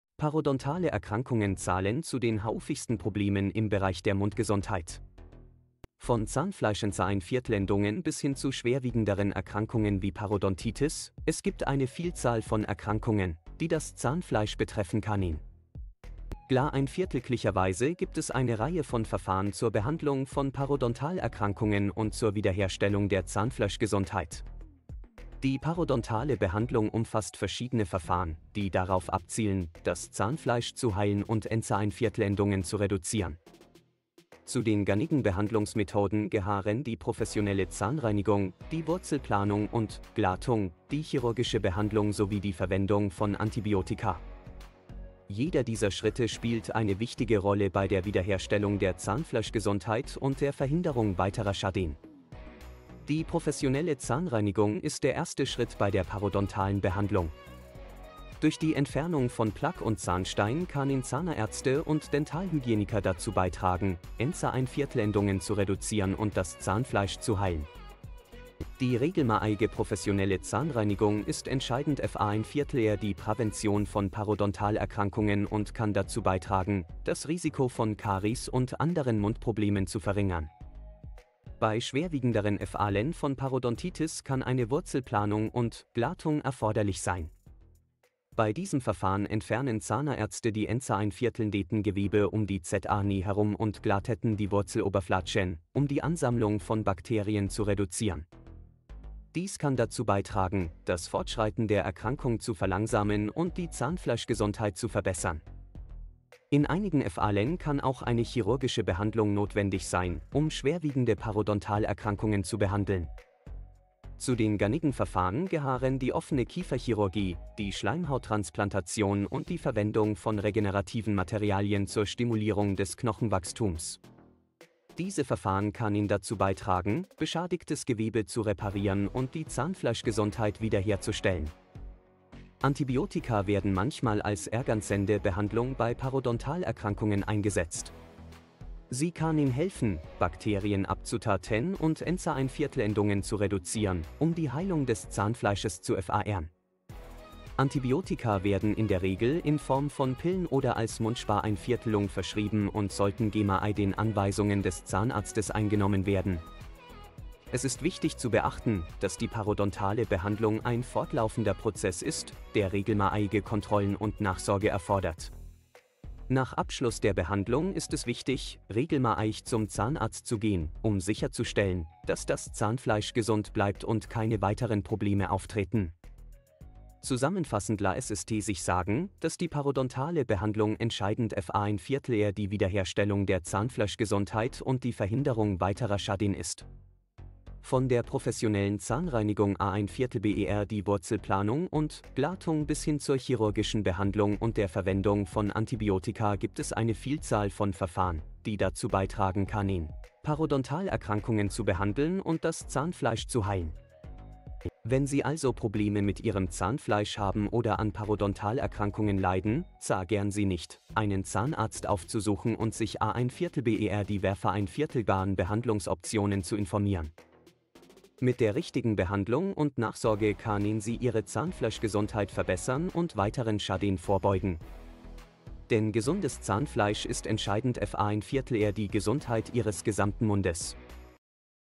Parodontale Erkrankungen: Wie kann man sie vermeiden und behandeln? - Welche Maßnahmen sind am
Parodontale Erkrankungen zählen zu den häufigsten Problemen im Bereich der Mundgesundheit. Von Zahnfleischentzündungen bis hin zu schwerwiegenderen Erkrankungen wie Parodontitis - es gibt eine Vielzahl von Erkrankungen, die das Zahnfleisch betreffen können. Glücklicherweise gibt es eine Reihe von Verfahren zur Behandlung von Parodontalerkrankungen und zur Wiederherstellung der Zahnfleischgesundheit. Zahnfleischentzündung, Parodontitis, Mundgesundheit, Parodontale Erkrankungen, Zahnfleisch, Ent